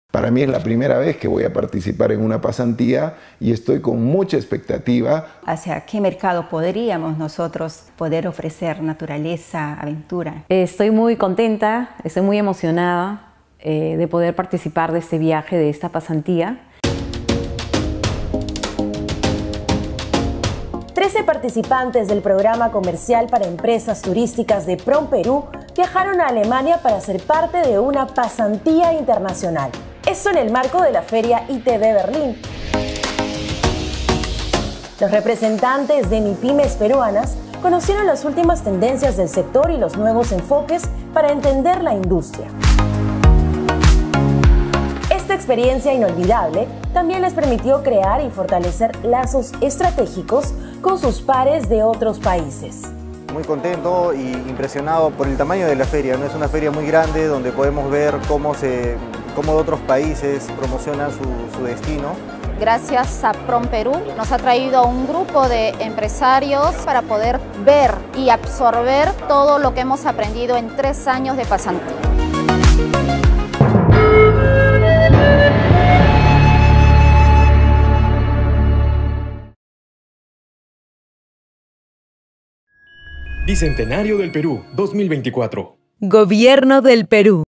Programa Comercial para Empresas Turísticas - Pasantía ITB Berlín 2023
Conoce las experiencias de los empresarios graduados del Programa Comercial para Empresas Turísticas que participaron en la pasantía internacional en la feria de turismo ITB 2023 en Berlín.